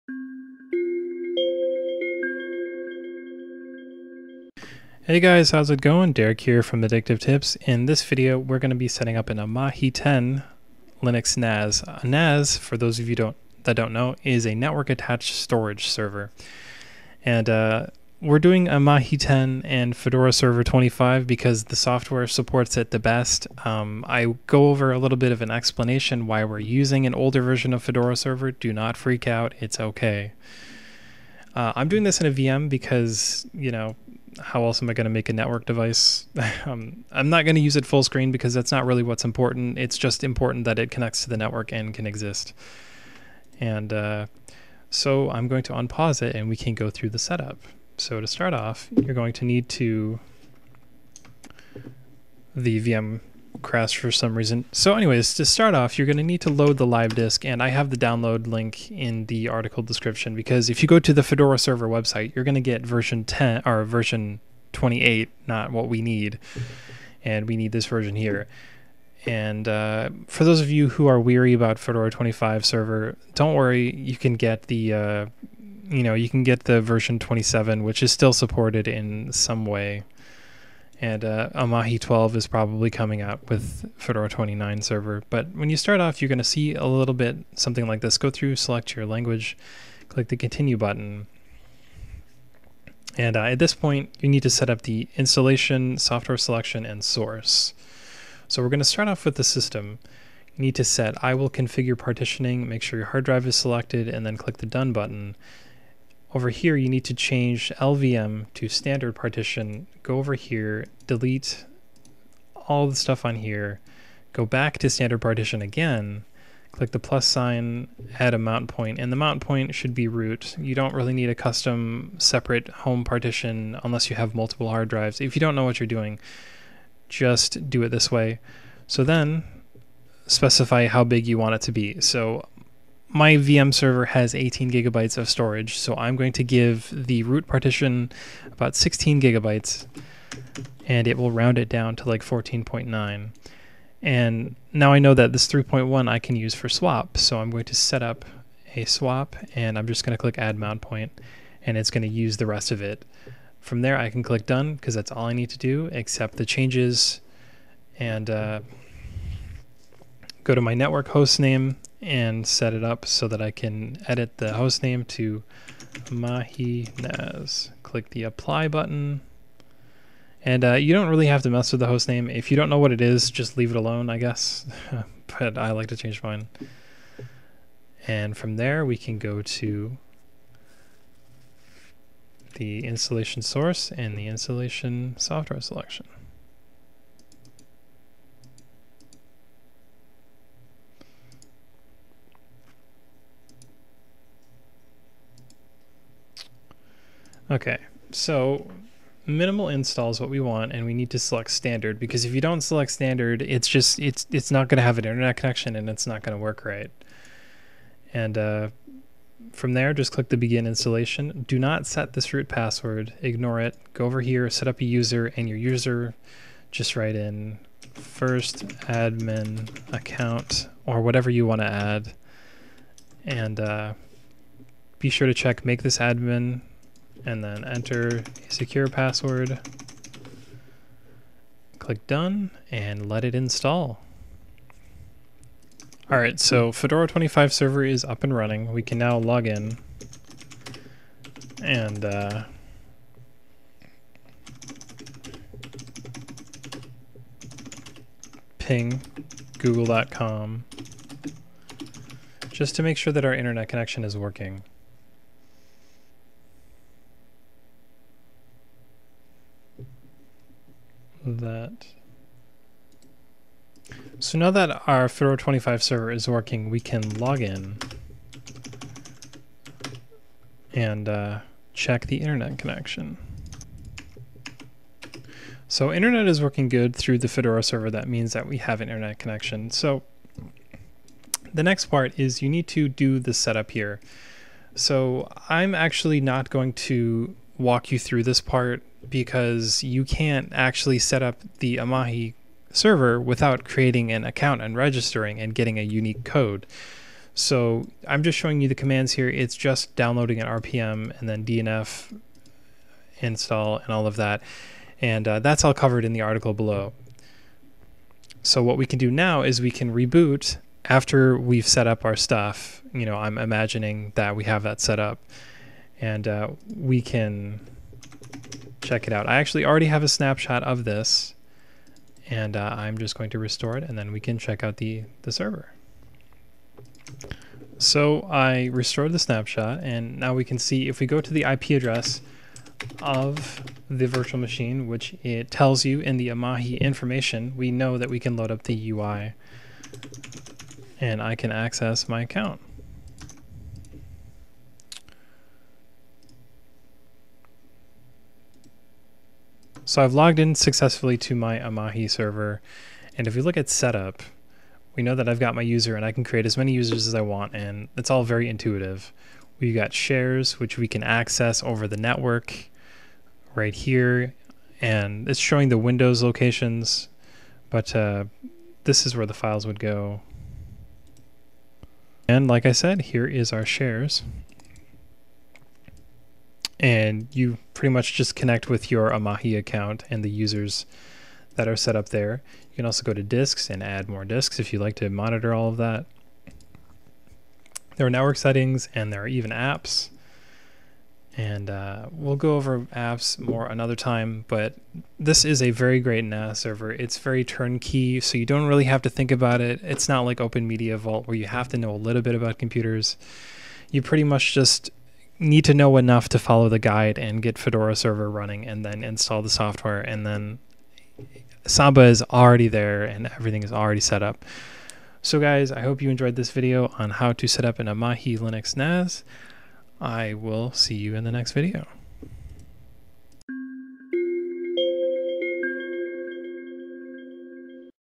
How To Create A Linux NAS With Amahi
Linux servers are complex and hard to set up, especially if you’re a new user. As a result, many projects have popped up to make having your own Linux file server much easier. One such project that attempts to make owning and operating a Linux NAS is Amahi.